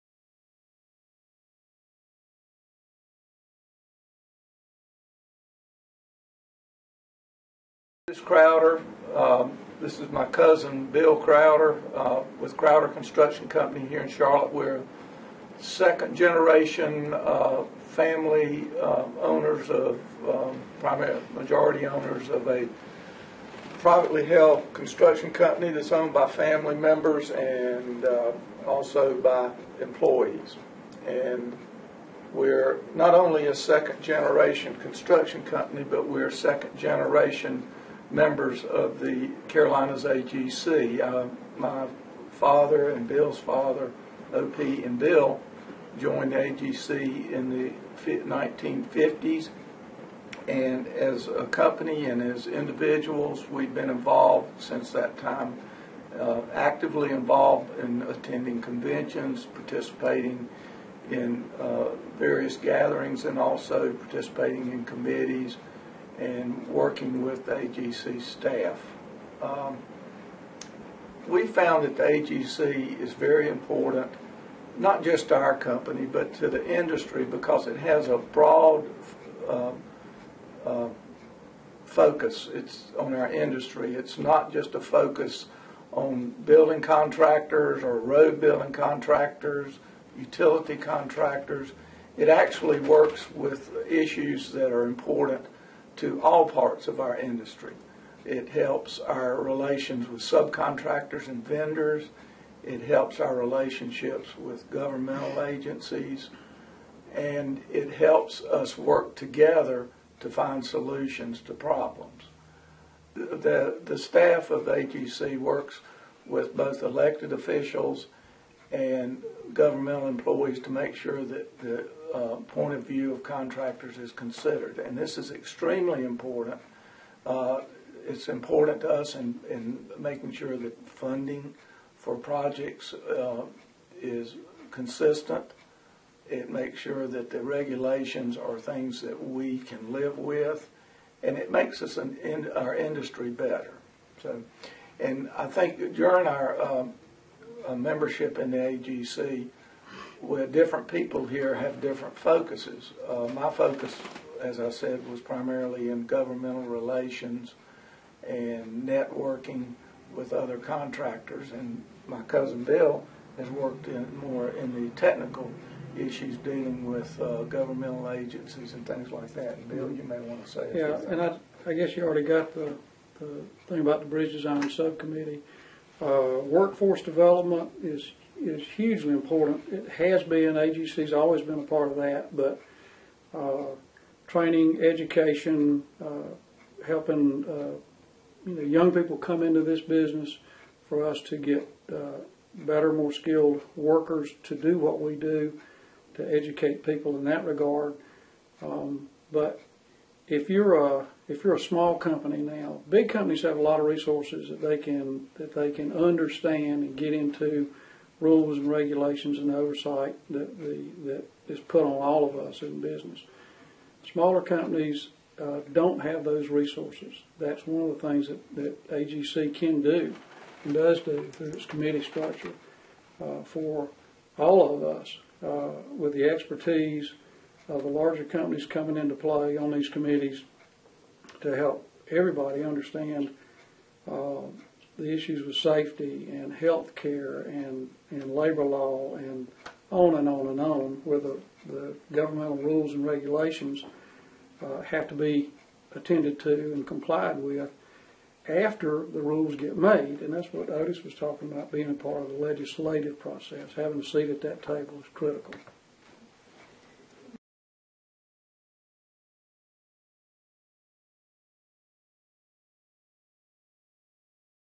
Straight from a member's mouth: Why Carolinas AGC membership matters
Bill and Otis Crowder of longtime Carolinas AGC member Crowder Construction share why membership is important to them - and you!